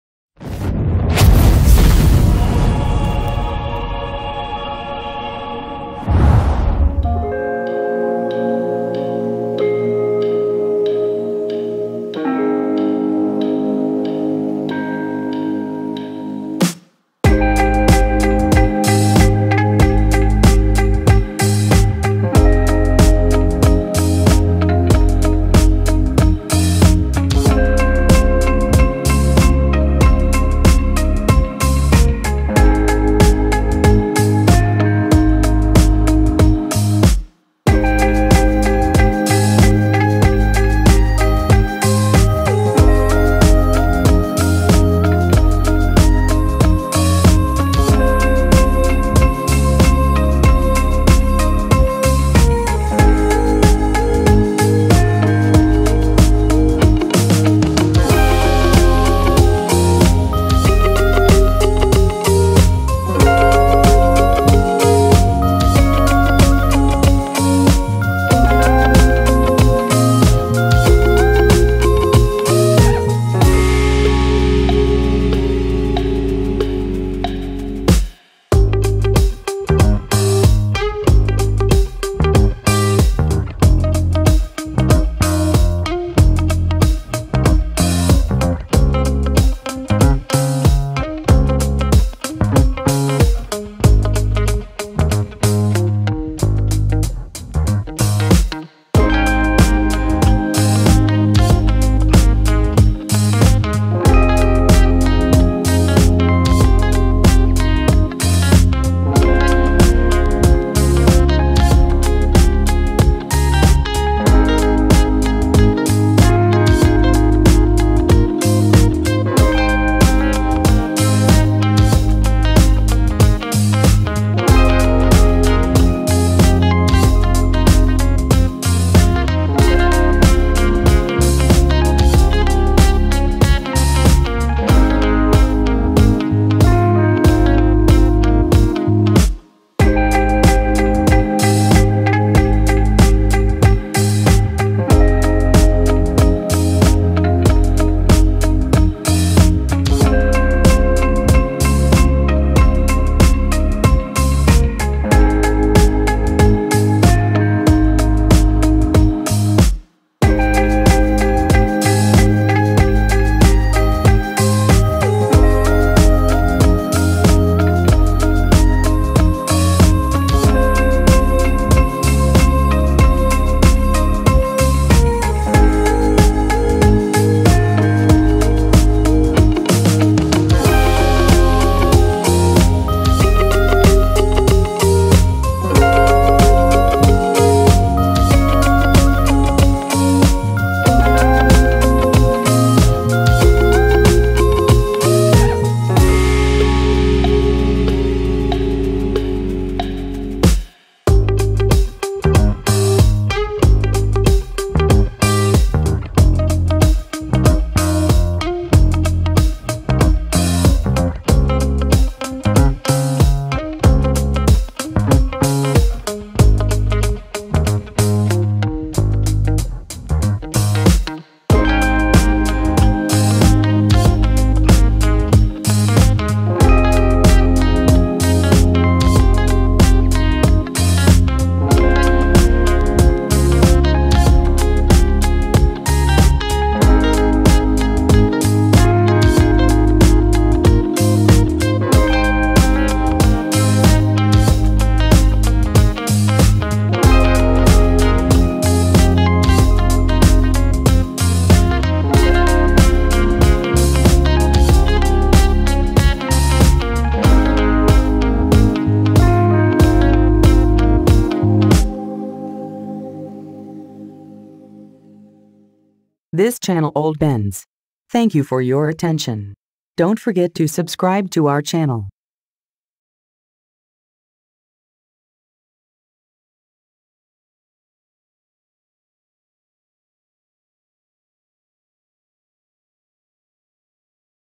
Mercedes-Benz 300 SEL 6.3 w109 the ancestor S-class 63 AMG
In March 1968 the 300 SEL 6.3 became top of the class of w108/w109 model series. It was equipped with the V8-engine and the automatic gearbox of the 600 models so that its power was equal to that of a high-level sportscar. Its presentation at the Geneva Motor Show created a sensation, especially since there had been no hints, that a model like this was to be forthcoming. From outside, the 6.3 could only be recognized by its broader tires, double halogen headlights, and additional distance beam headlights. Even though it cost over DM 10.000 more than a 300 SEL and twice as much as the 280 SE, many customers were interested in the 300 SEL 6.3, so that 6,526 units were produced.

source: AutoSL, autosl.de